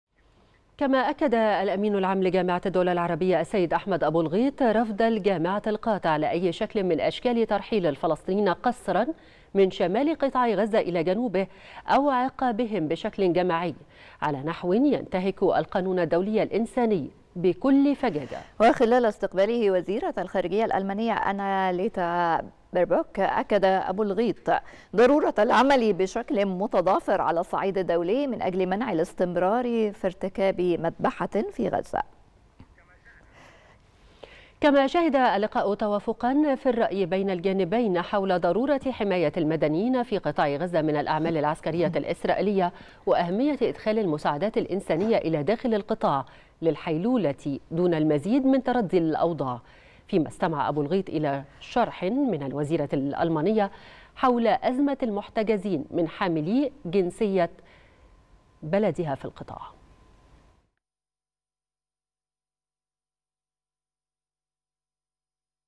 أبو الغيط: الجامعة العربية ترفض رفضاً قاطعاً ترحيل الفلسطينيين قسراً من شمال قطاع غزة إلى جنوبه
نشرة الثالثة | أبو الغيط: الجامعة العربية ترفض رفضاً قاطعاً ترحيل الفلسطينيين قسراً من شمال قطاع غزة إلى جنوبه